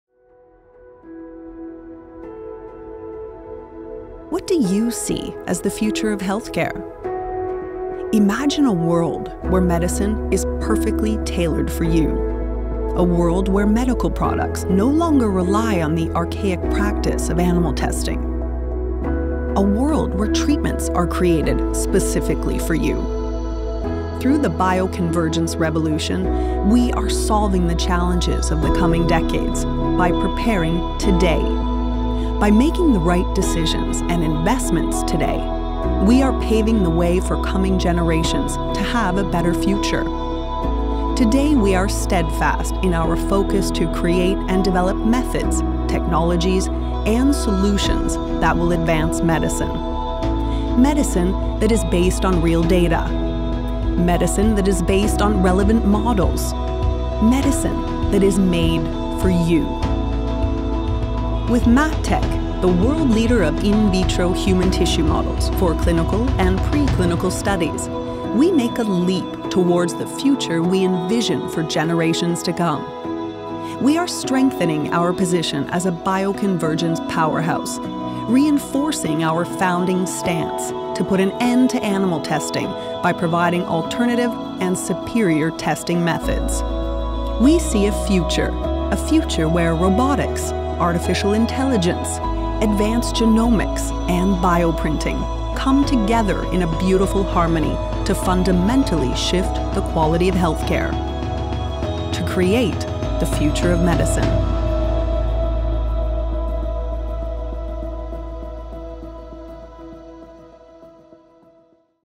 CELLINK welcomes MatTek
CELLINK acquires MatTek, a global leader in advanced in-vitro technology for clinical and pre-clinical studies strengthening the overall offering of alternative models for drug testing and thus reducing the dependency on animal models.